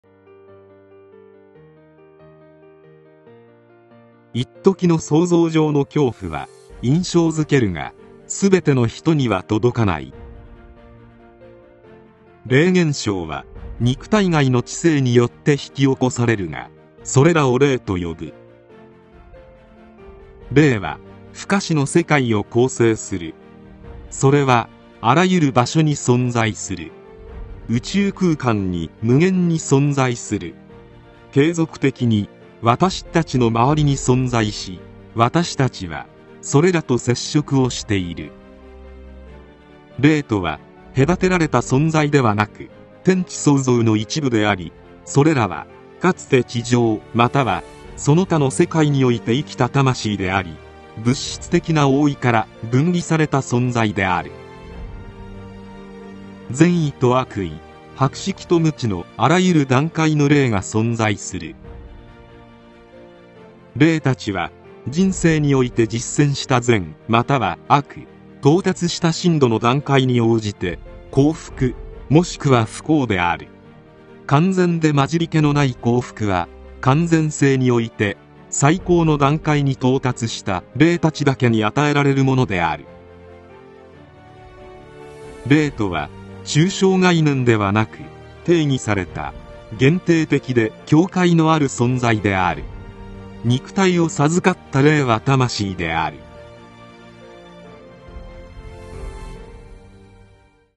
LDM 8-14 🇯🇵 Giapponese
160 Anni de "Il Libro dei Medium" - In occasione dell’anniversario della pubblicazione del libro, il Consiglio Spiritista Internazionale lo celebra pubblicando ogni settimana un nuovo video, in cui vengono letti alcuni brani. Il video viene pubblicato in 13 lingue diverse, a testimoniare il valore mondiale dell'opera di Kardec.